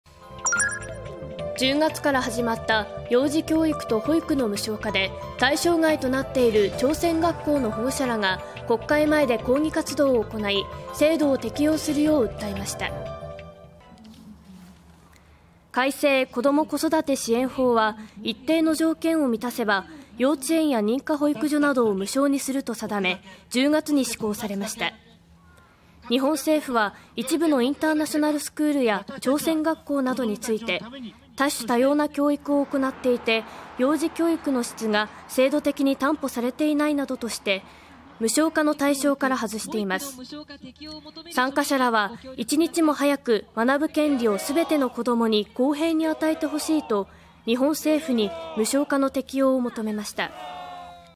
幼保無償化求め朝鮮学校の保護者らが国会前で抗議(19/12/06)
10月から始まった幼児教育と保育の無償化で、対象外となっている朝鮮学校の保護者らが国会前で抗議活動を行い、制度を適用するよう訴えました。

　改正「子ども・子育て支援法」は一定の条件を満たせば、幼稚園や認可保育所などを無償にすると定め、10月に施行されました。日本政府は一部のインターナショナルスクールや朝鮮学校などについて「多種多様な教育を行っていて、幼児教育の質が制度的に担保されていない」などとして無償化の対象から外しています。参加者らは、「一日も早く、学ぶ権利をすべての子どもに公平に与えてほしい」と日本政府に無償化の適用を求めました。